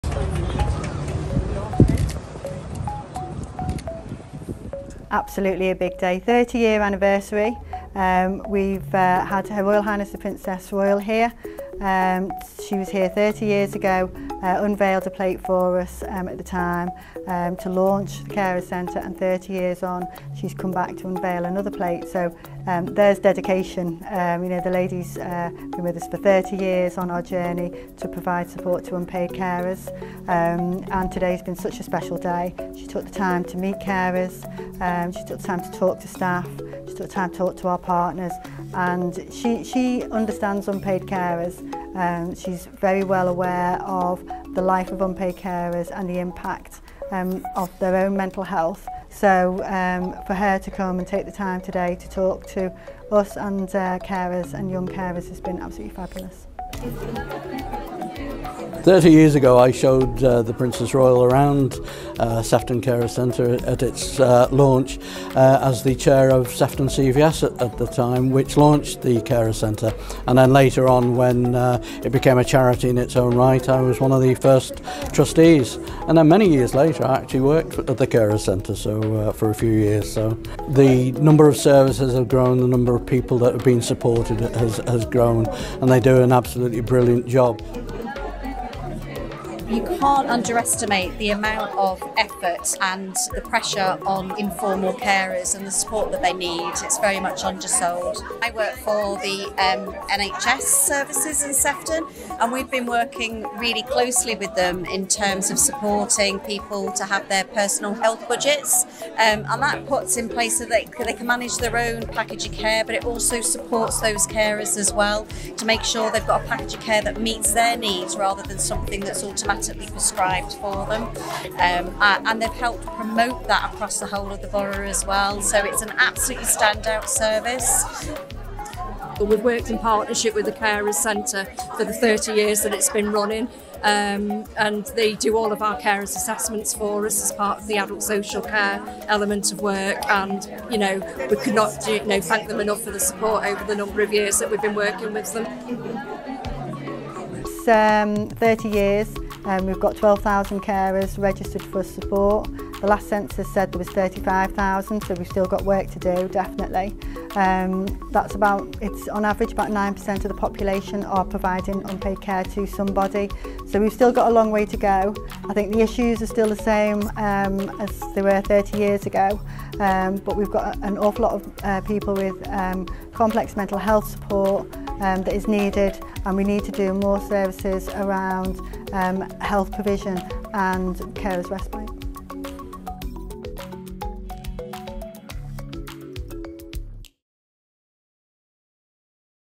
HRH Princess Royal's visit to Sefton Carers Centre
On Tuesday 16th April 2024, HRH Princess Royal visited to Sefton Carers Centre to mark its 30th anniversary.
The visit highlighted the work carried out by Sefton Carers Centre over the past 30 years and emphasised the organisation's ongoing commitment to enhancing the well-being of carers to care for a family member, friend, or neighbour.